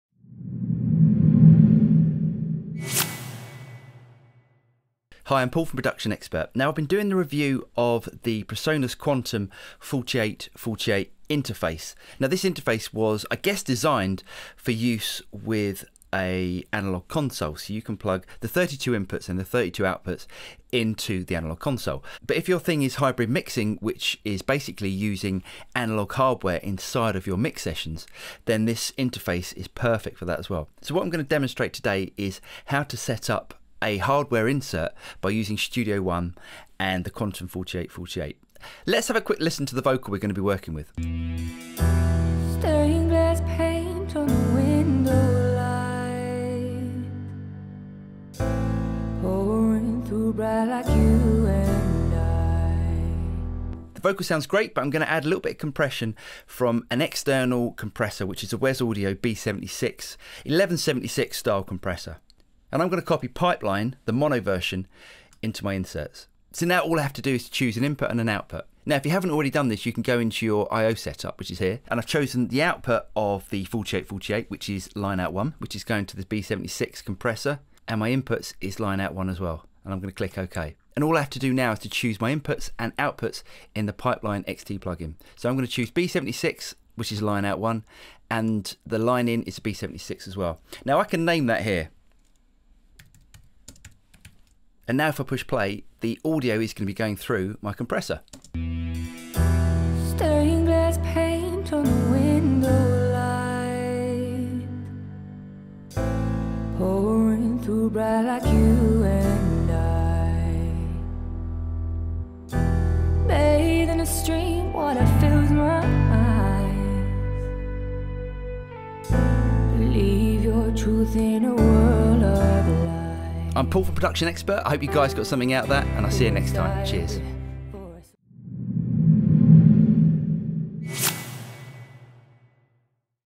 PreSonus Quantum 4848 Thunderbolt Interface
The Quantum 4848 is a 48 in and 48 out interface with 32 analog in and outs and 16 ADAT in and outs.